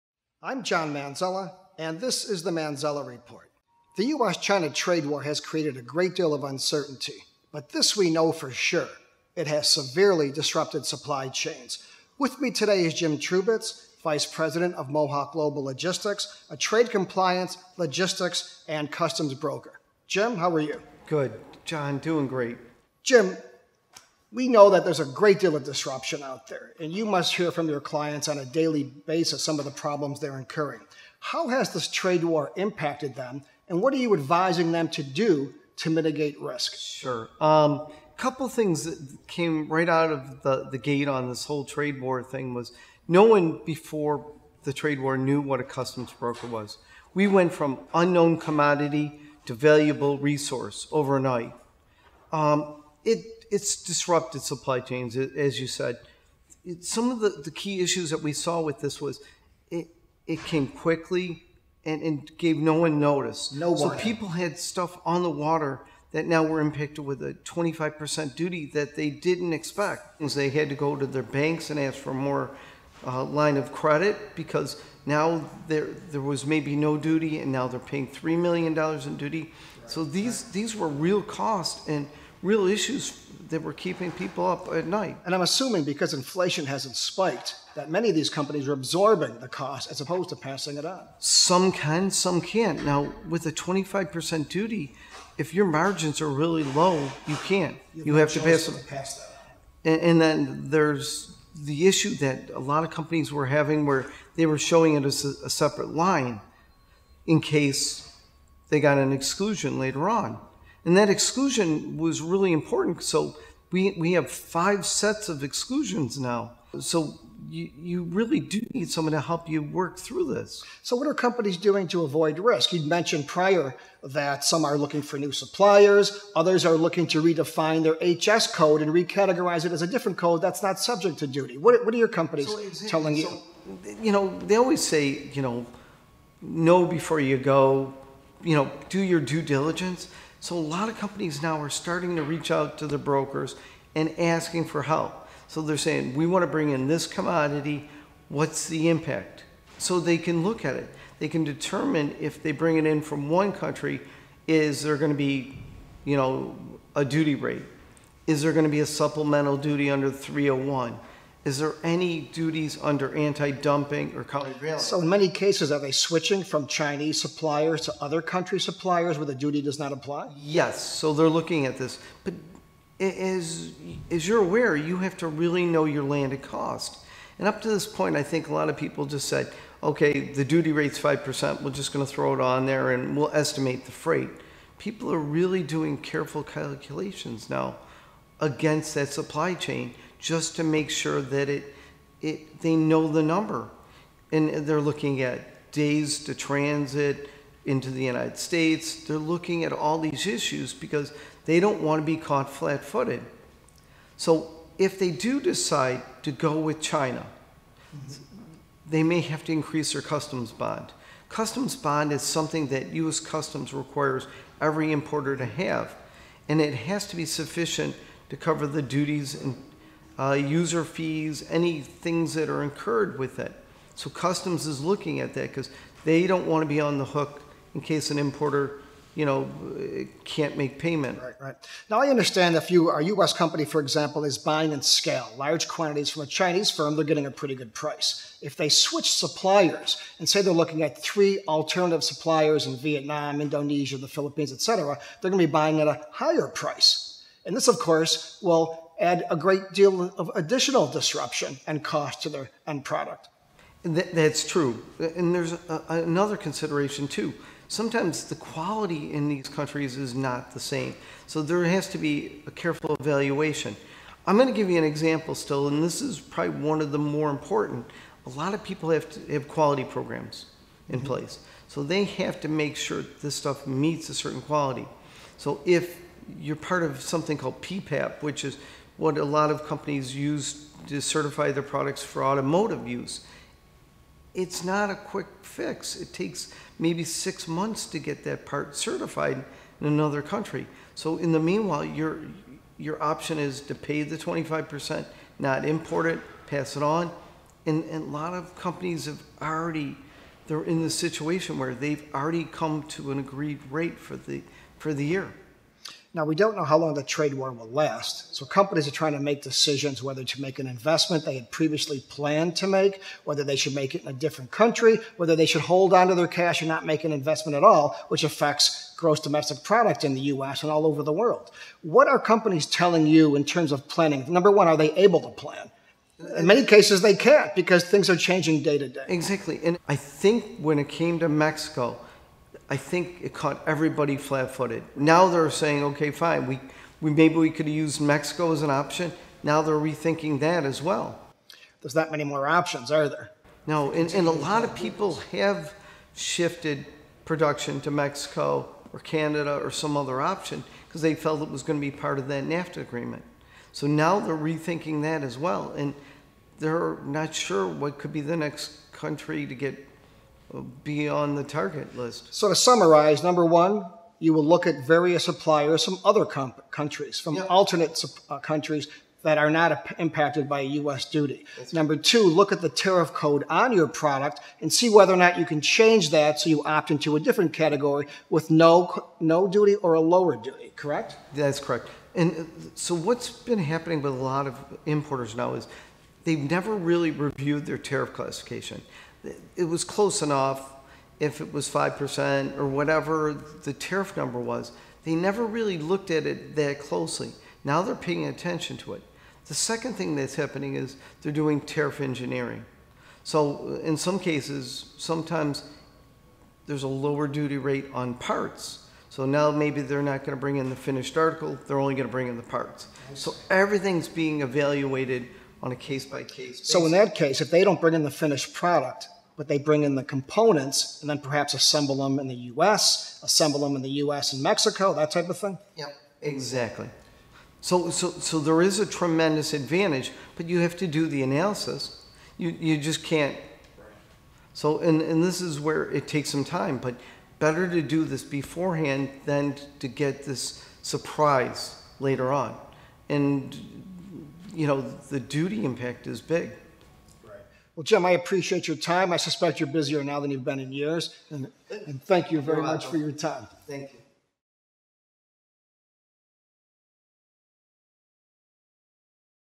China Trade War: What To Do
A Manzella Report interview with Jim Trubits, Vice President, Mohawk Global Logistics, discusses the disruptions and costs of the U.S.-China trade war and what companies can do to reduce risks.